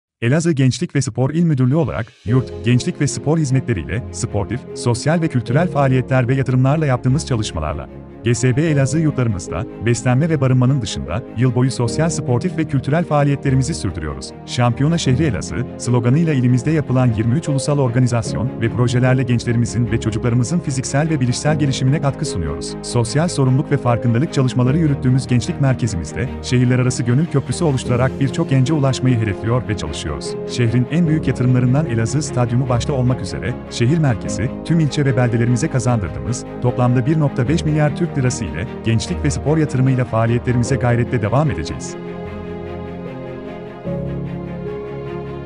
ELAZIĞ GENÇLİK VE SPOR İL MÜDÜRLÜĞÜ - ANADOLUNUN ENLERİ ÖDÜL TÖRENİ VTR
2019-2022 yılları arasında gerçekleştirdiği bir çok faaliyet ve Yatırımlarla, Anadolu'nun Enleri Ödül Törenin'de, ödüle layık görülen Elazığ Gençlik ve Spor İl Müdürlüğü adına ödül alan İl Müdürü Abdulsamet Eren adına BKS Medya ve Mart Medya'ya şükranlarımızı sunuyoruz.